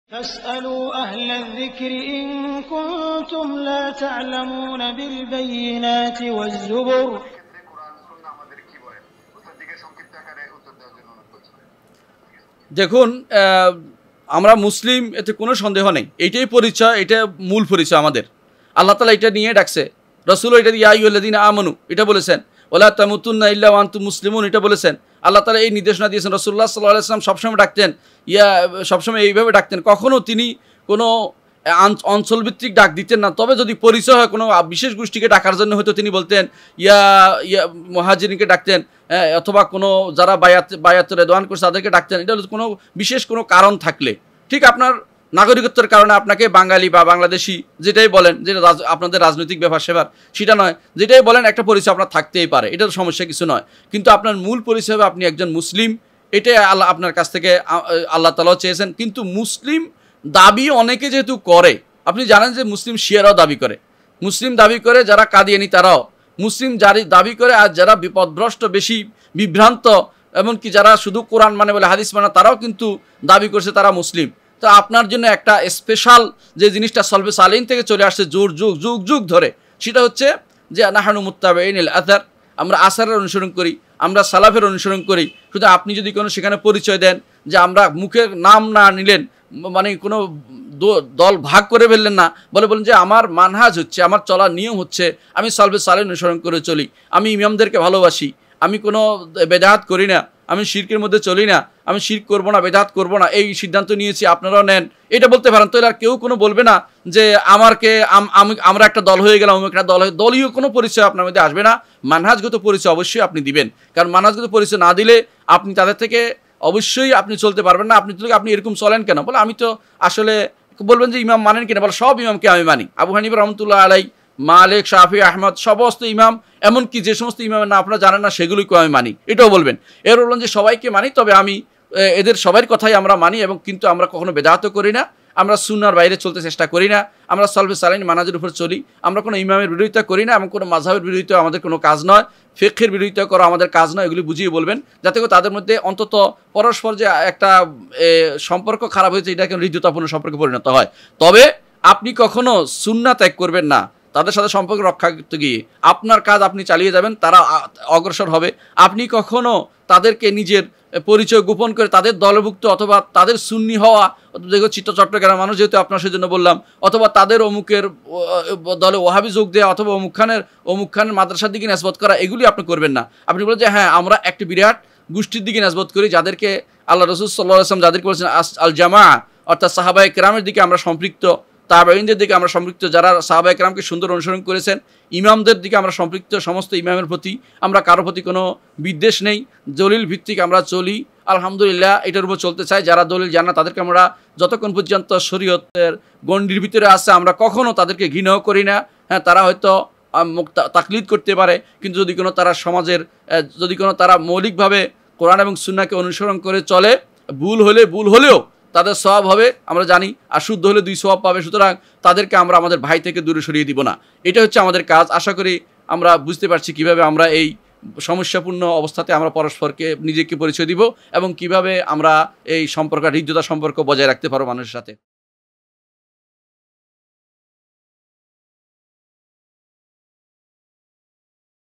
প্রশ্ন : সবাই মুসলিম বলে আমাদের করণীয় কি? শাইখ প্রফেসর ড. আবু বকর মুহাম্মাদ যাকারিয়া
প্রশ্ন : সবাই মুসলিম বলে আমাদের করণীয় কি?
শাইখ প্রফেসর ড. আবু বকর মুহাম্মাদ যাকারিয়া